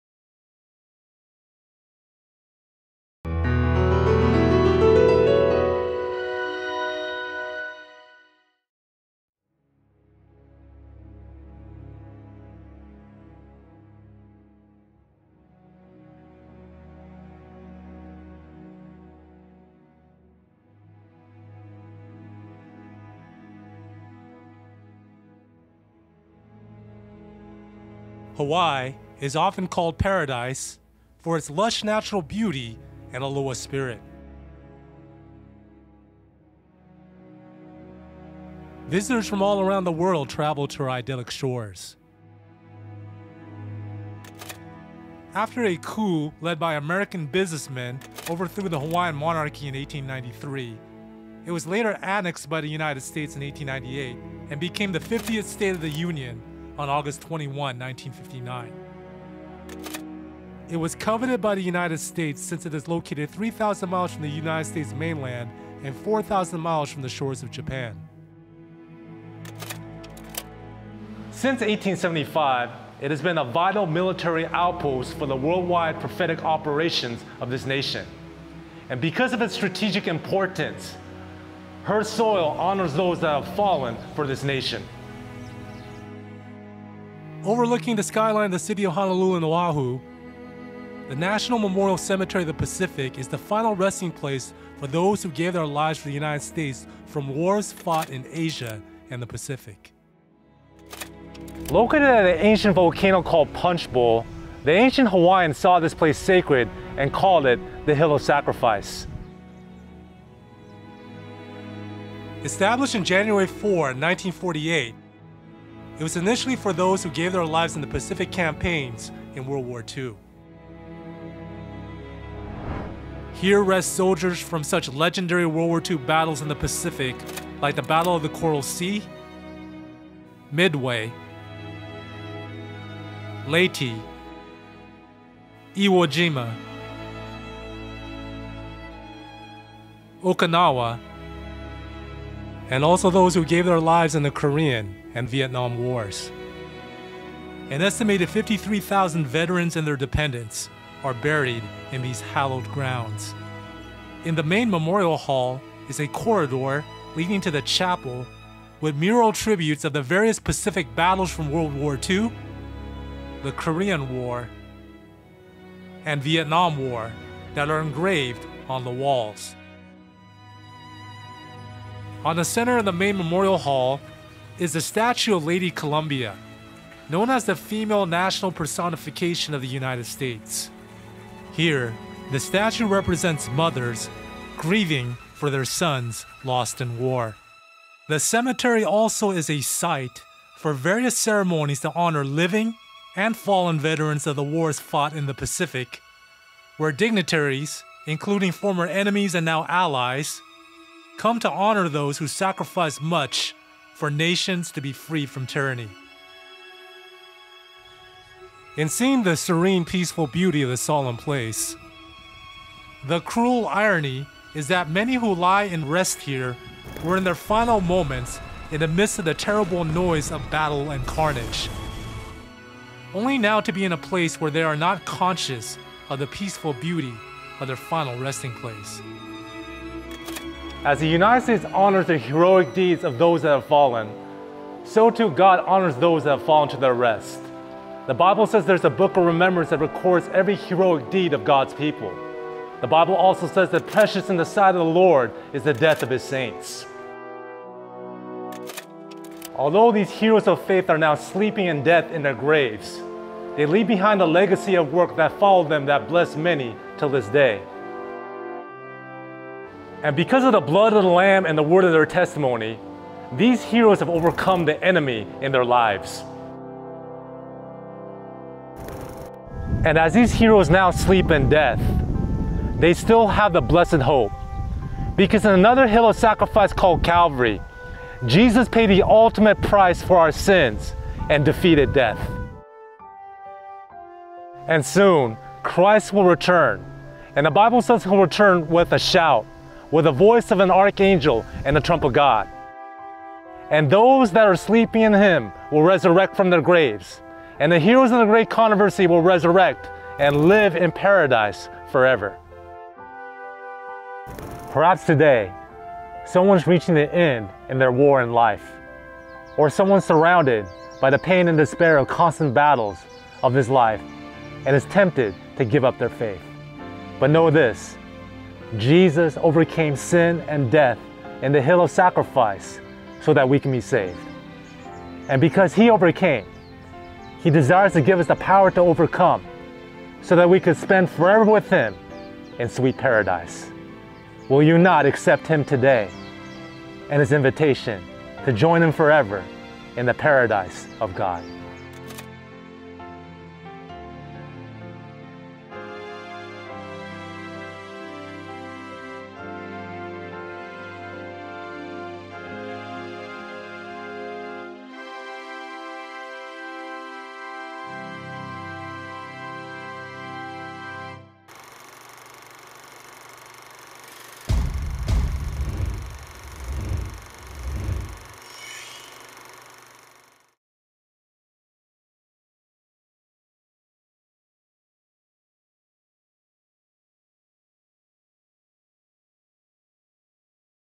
"The Hill of Sacrifice." (The History of Punchbowl National Memorial Cemetery) EPISODE 11
The National Memorial Cemetary of the Pacific, known as punchbowl is located in Honolulu, Hawaii, and is the final resting place for those who gallantly gave their lives for the United States in World War II, The Korean War, and the Vietnam War.  It is called the Hill of Sacrifice. This episode will explore the history of the cemetery and powerful object lessons on another hill of sacrifice, death was conquered, and a resurrection unto eternal life is promised.

🎵 Ben M. Evardome